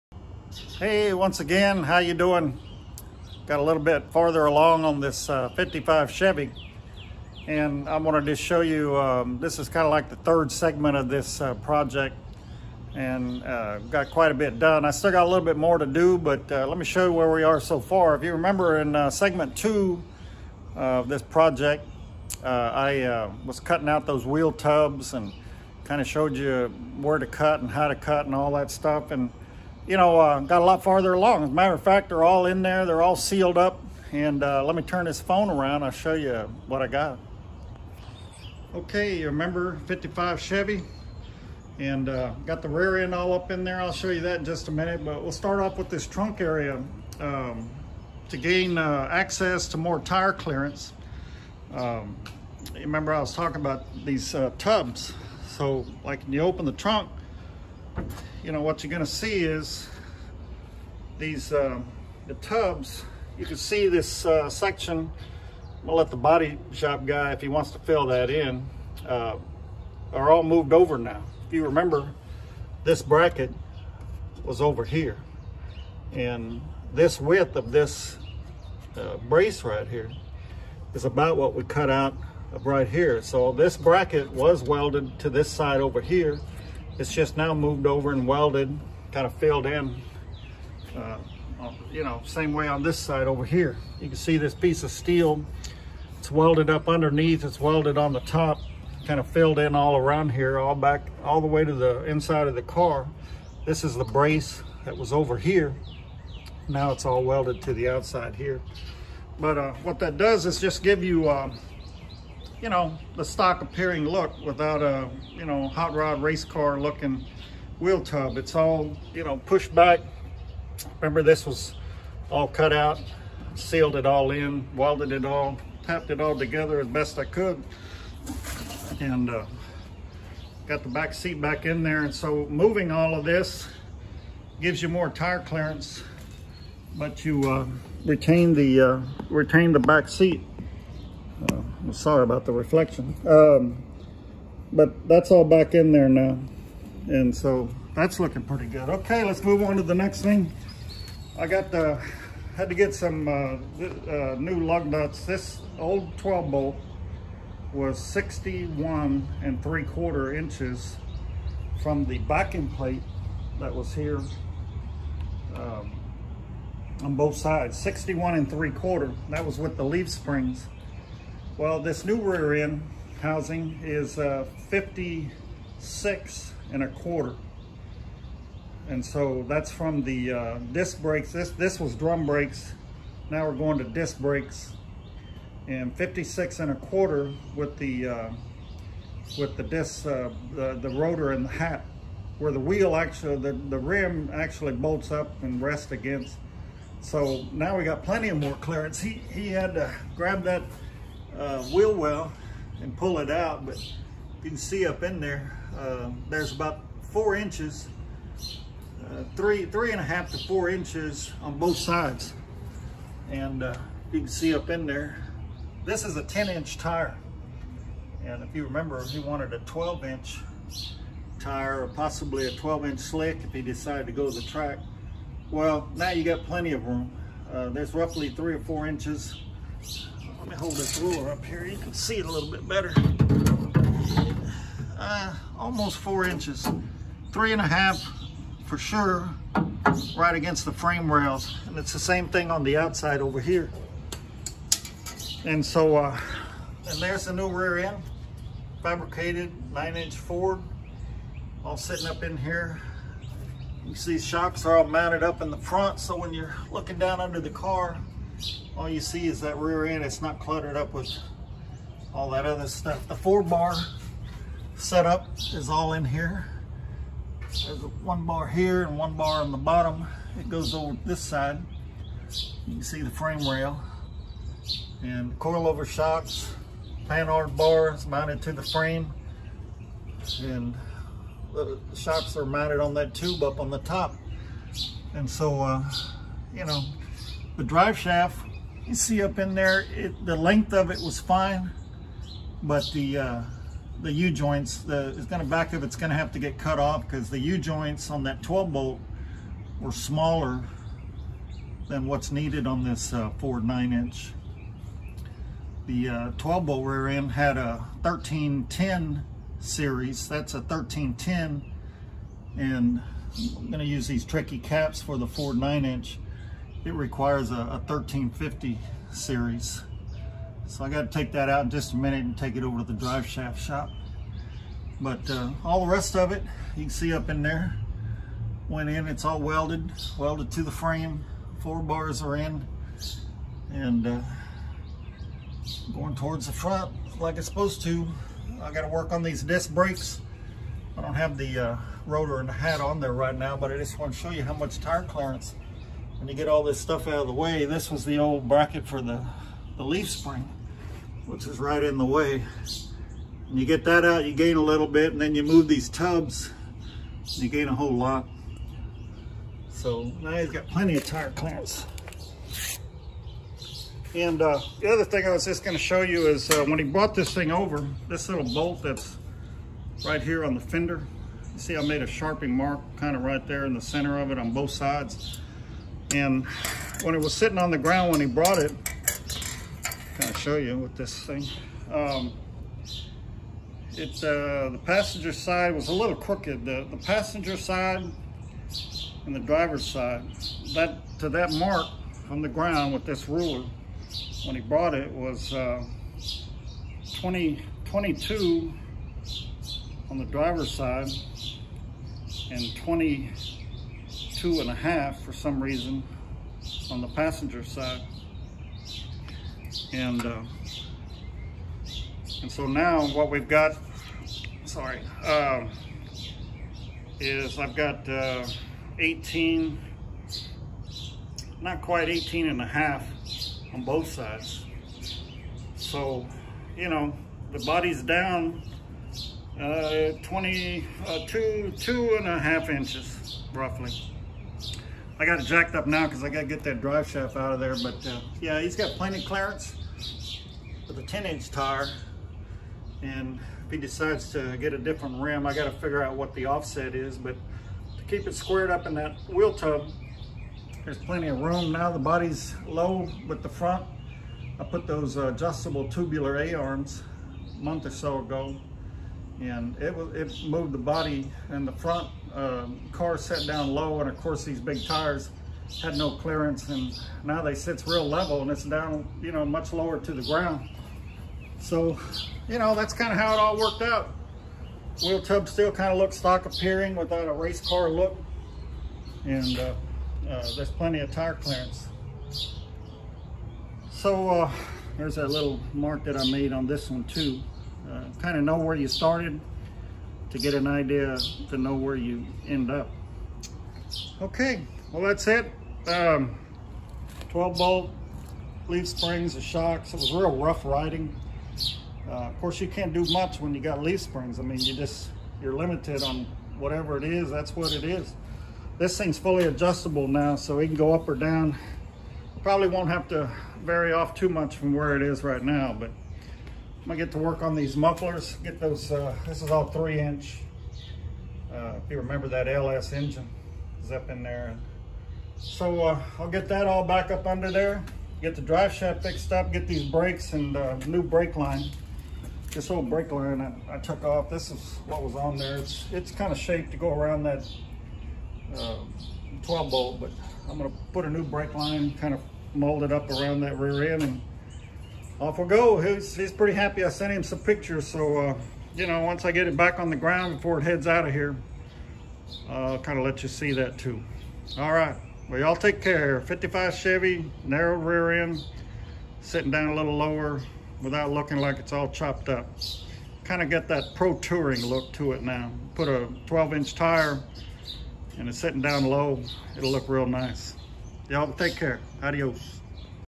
1955 Chevy 3rd Segment
The third segment on narrowing and lowering the rear end of a 1955 Chevrolet